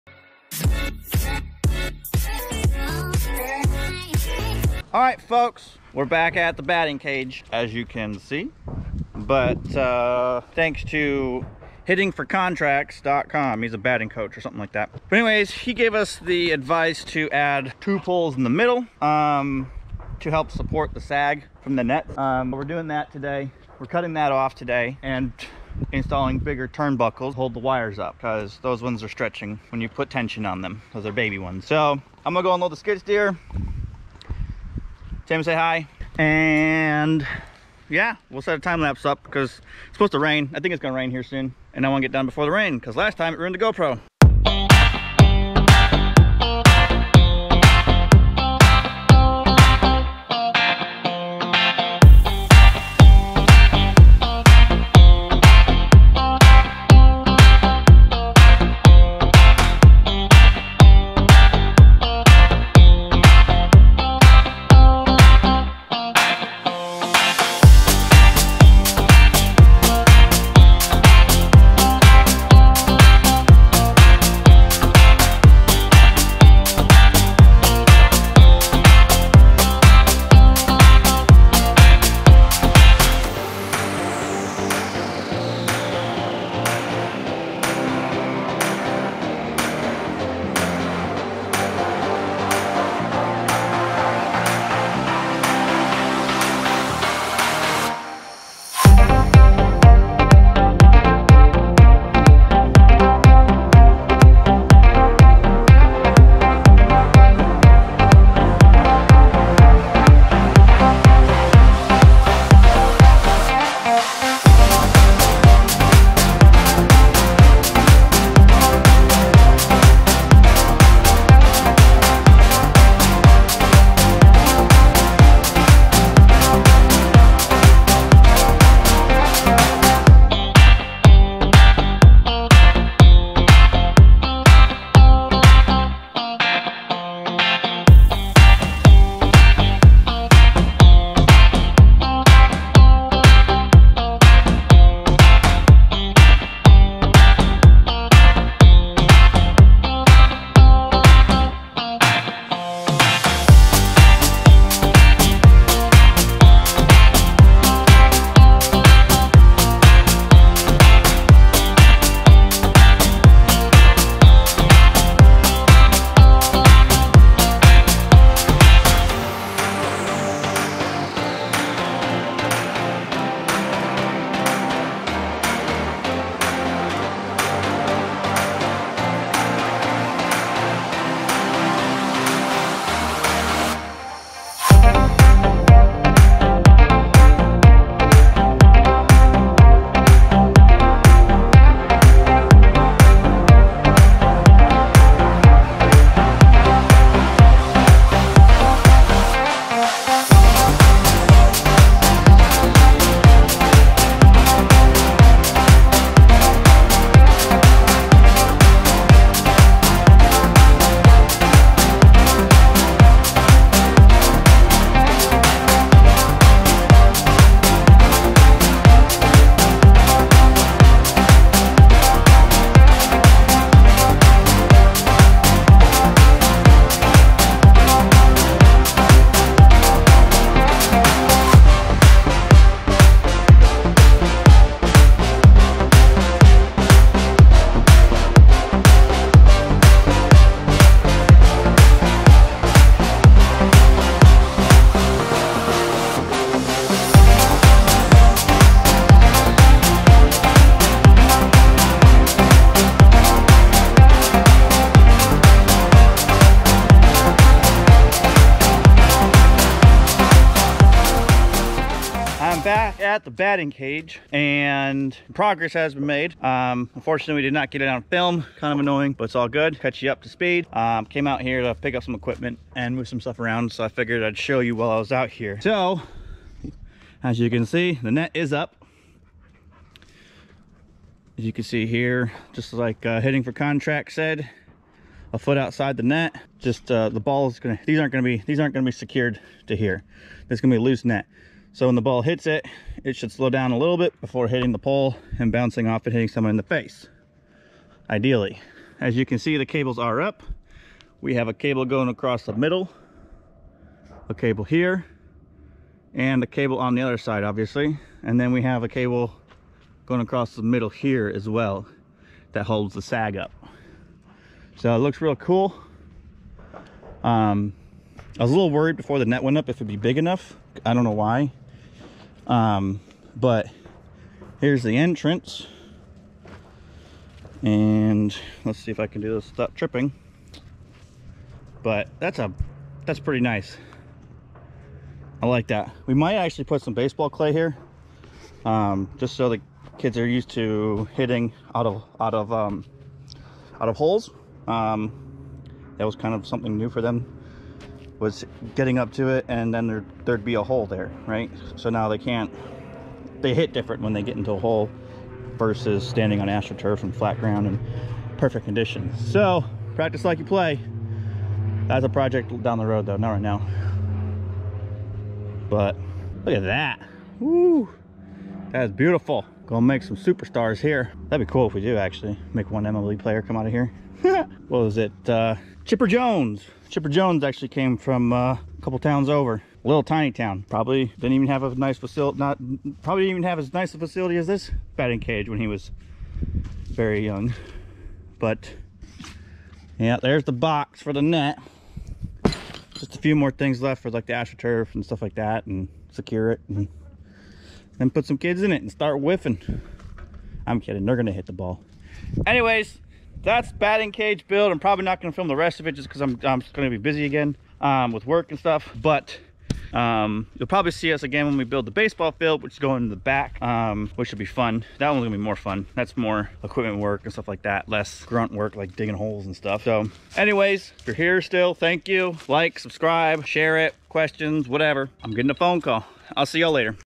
Building a Batting Cage Part 2
Went back and added two more poles in the middle thanks to advice from hitting4contracts.com. This will help support the weight of the net.

Net also goes up in this video. Turf time and then its time to bring the players in to start batting practice.

Should be fun.

Follow along on the other Social Media for sneak peaks and all that.